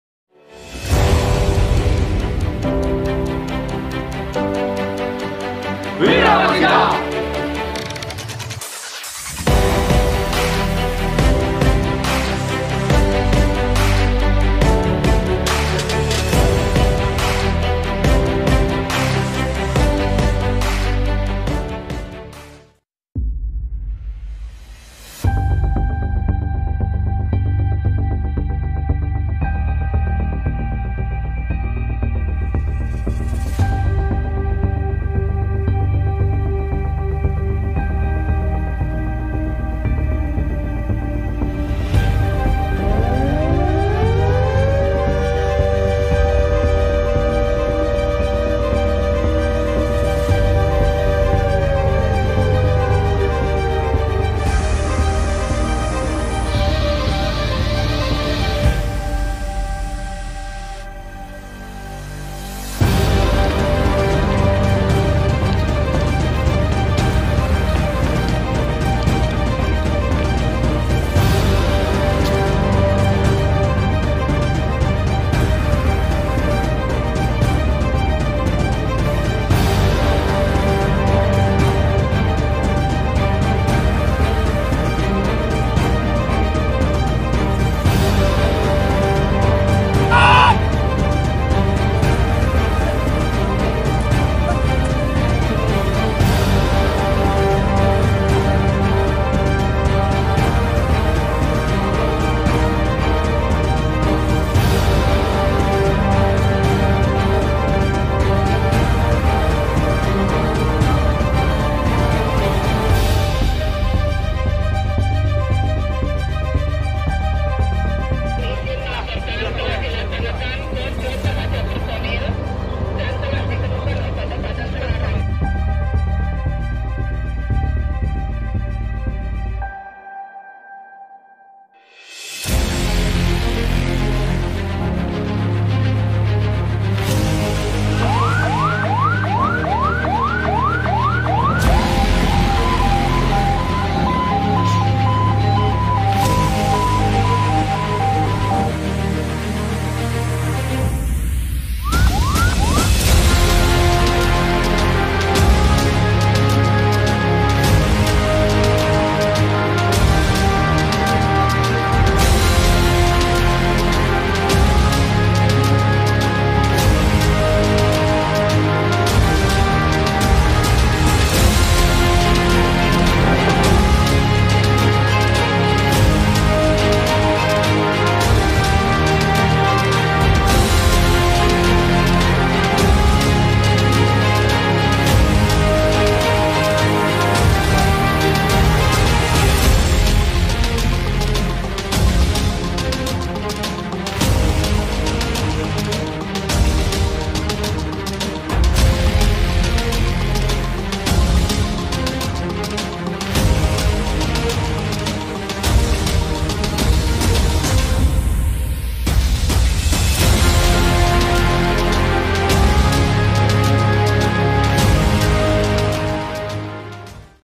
Latihan Garuda Perkasa Unsur Pomau Tahun 2022 oleh Satpom Lanud Abdulrachman Saleh
Latihan Garuda Perkasa Unsur Pomau oleh Satpom Lanud Abdulrachman Saleh ini bertujuan untuk melatih atau menguji seluruh kesiapan baik personel, materil maupun prosedur tetap khususnya pada unsur satpomau dalam mendukung operasi Lanud Abd Saleh.